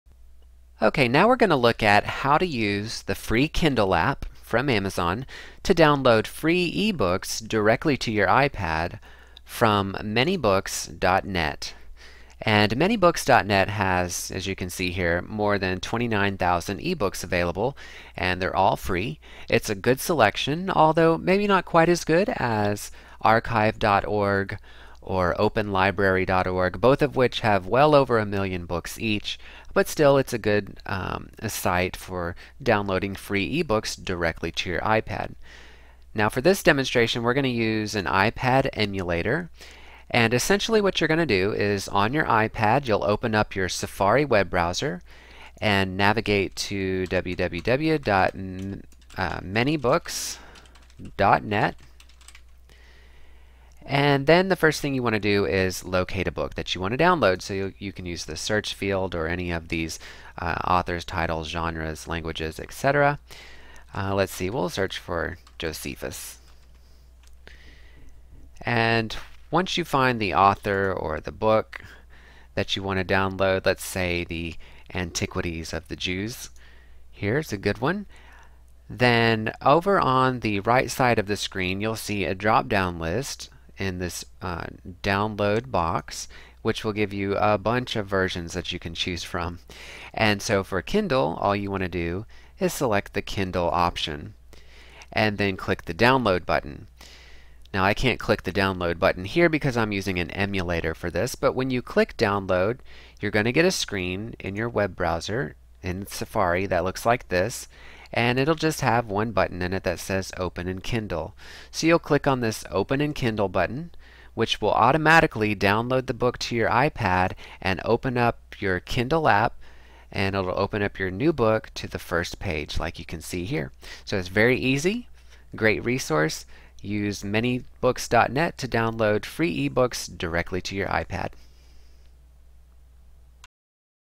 Download free books from ManyBooks.net to Kindle on iPad
How to download free eBooks from ManyBooks.net directly to the Amazon Kindle app on your iPad